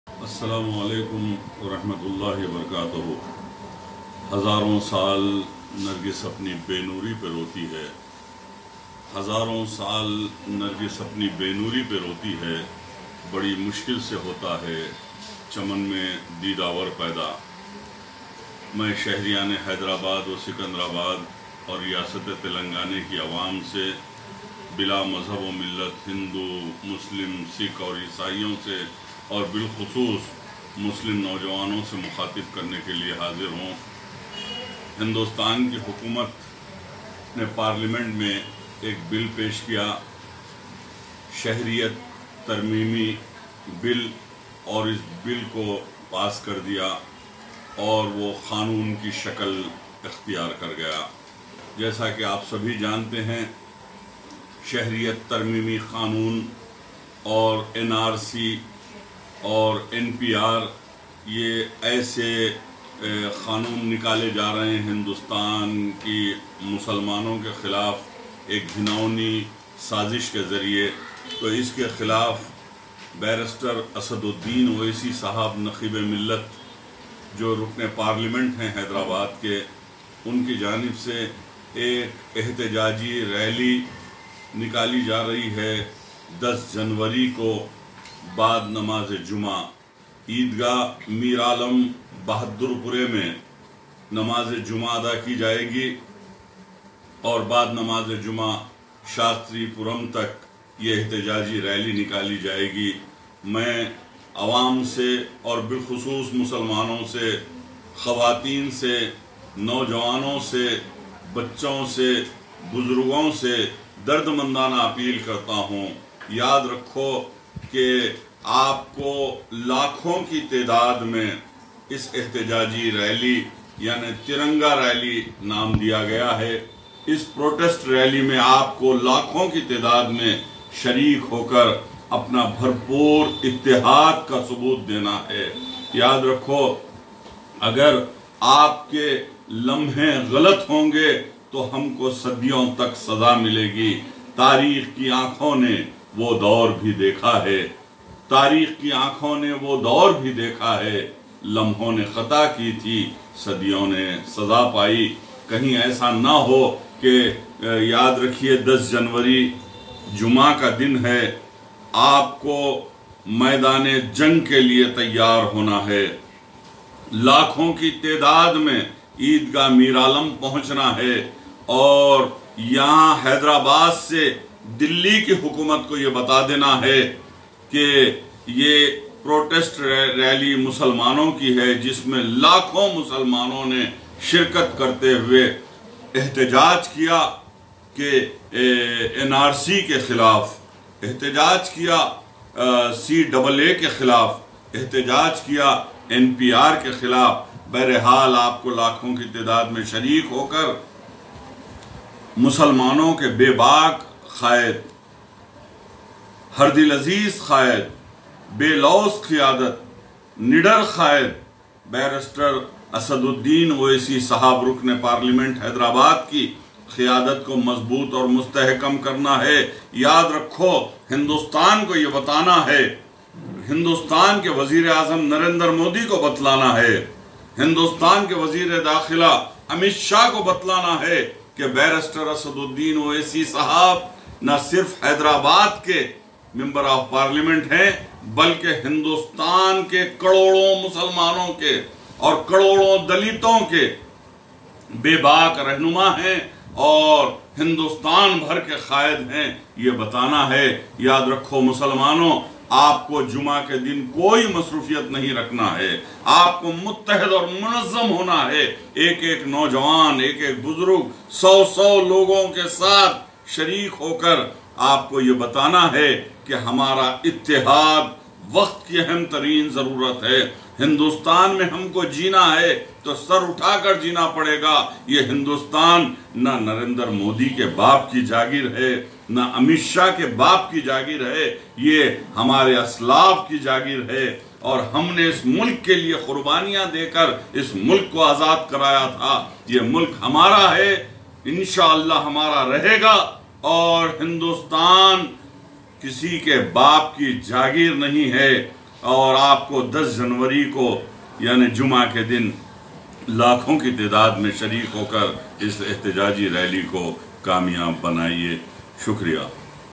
Mohd.Raheemullah Khan Niyazi TRS Senior Leader Appeals To Make Grand Success Eidgah Miralam Protest
SHER-E-TELANGANA Mohd.Raheemullah Khan Niyazi TRS Senior Leader & Ex Chairman Urdu Academy appealed to public especially muslims of hyd & telangana to Attend protest rally against NRC/CAA/NPR & make it grand success on 10th Jan 2.pm from Eidgah Miralam to Shastripuram..